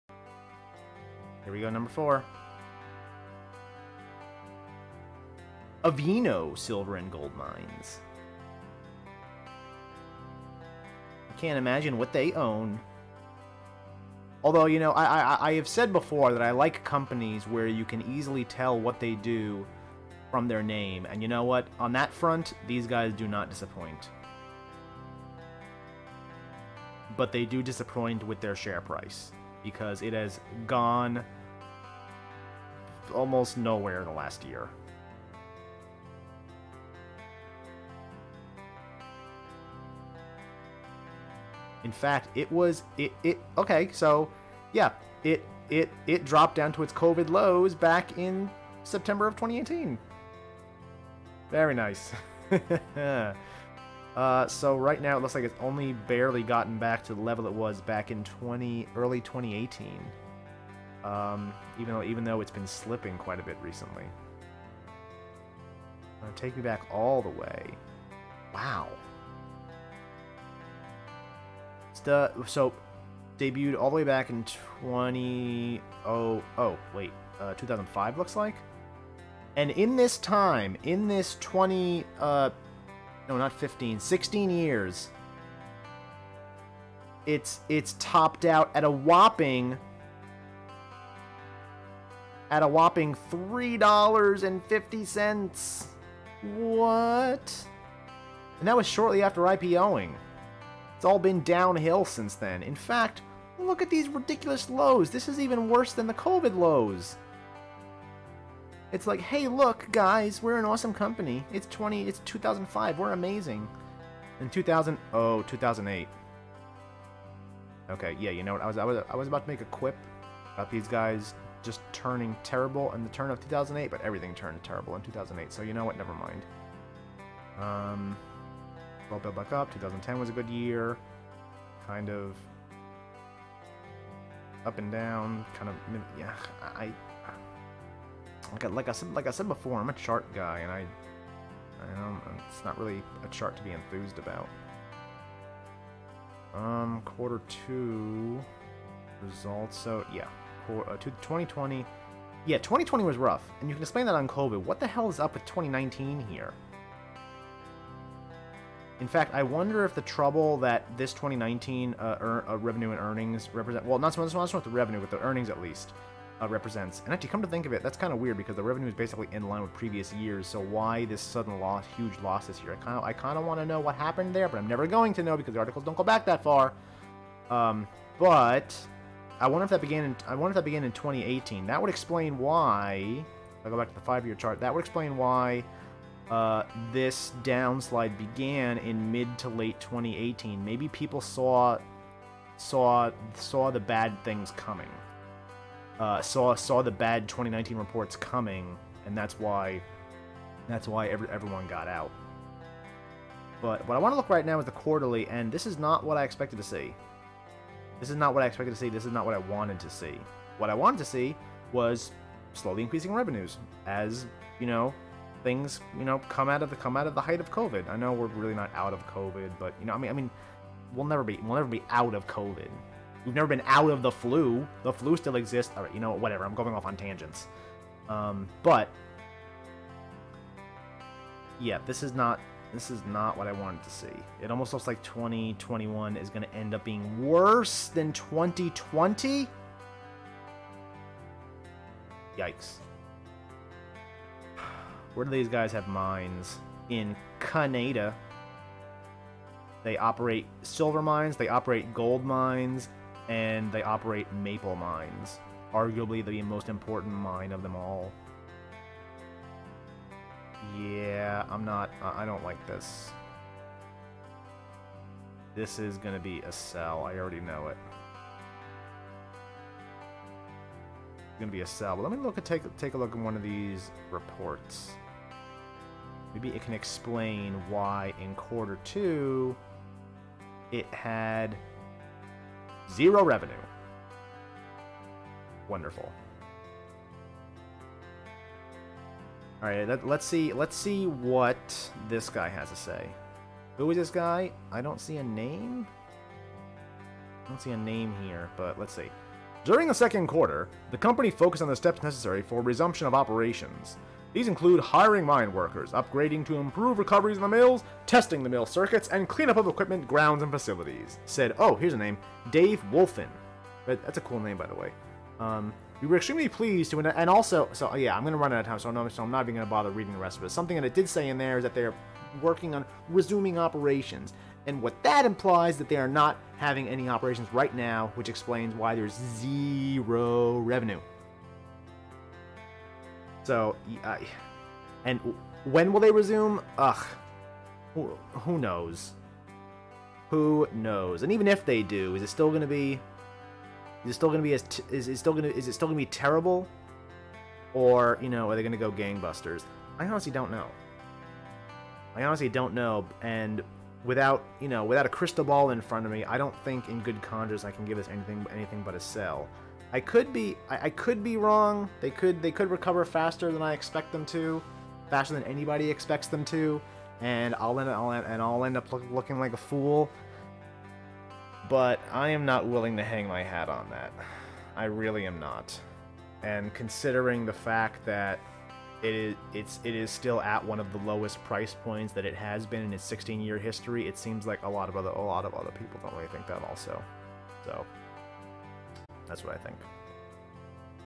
Speedrun of Avino Silver & Gold Mines (ASM)
What is a Stock Pick Speedrun? It's 5 minutes for me to decide if a random stock is a buy, sell, or hold! This run took place during the September 12, 2021 session.

So, how did I do? Let me know!

This video is part of the Original Series, which was streamed on Twitch. Series II is made specifically for YouTube with a longer, more stylized format!